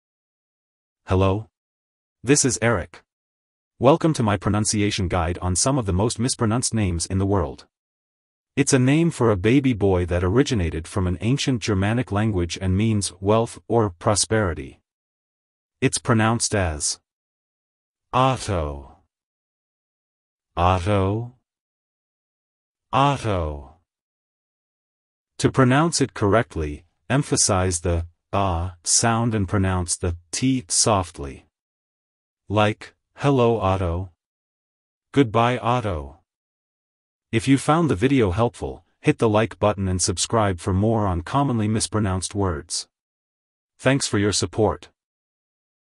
How to Pronounce Name Otto (Correctly!)
Welcome! Here you will hear how to pronounce Otto correctly.
It's a name for a baby boy that originated from an ancient Germanic language and means 'wealth' or 'prosperity'.
To pronounce it correctly, emphasize the 'ah' sound and pronounce the 't' softly.

b648